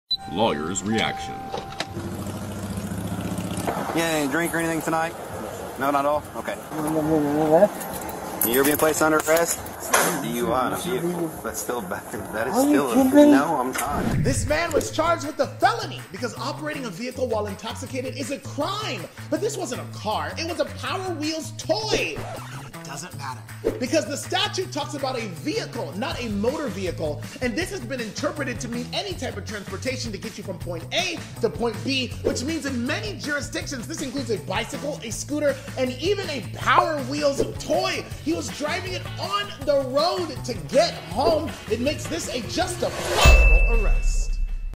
Man gets a DUI while driving a children’s toy Power Wheels Jeep. Attorney Ugo Lord reacts! ￼
Drunk man drives a battery powered children’s power wheel toy jeep on the road and gets charged with drinking and driving! In Indiana this is charged as operating while intoxicated (OWI)!